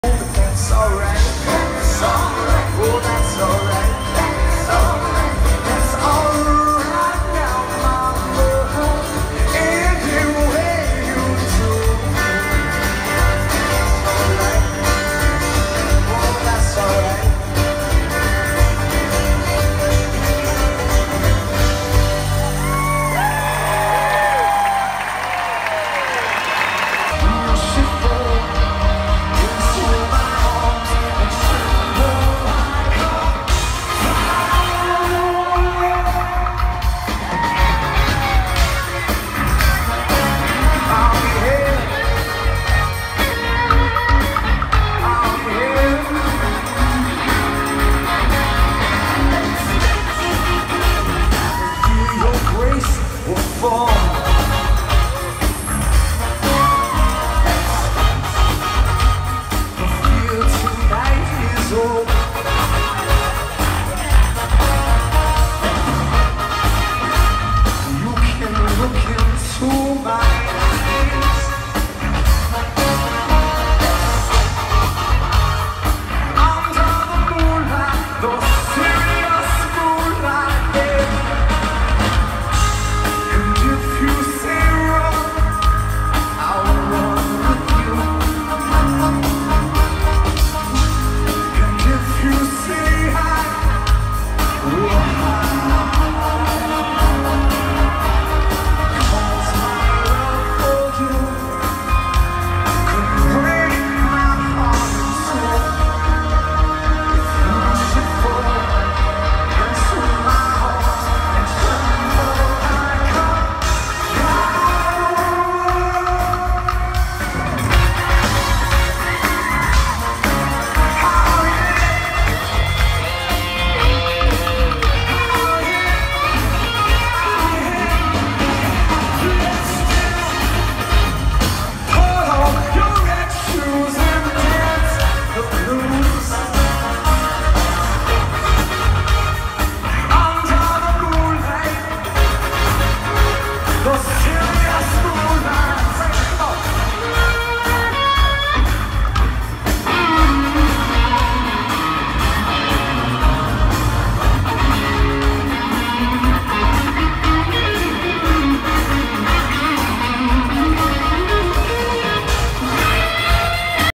Rob Thomas covering Elvis & Bowie @ Lakeview Amphitheater, Syracuse, NY 8/12/16.
Matchbox Twenty lead man.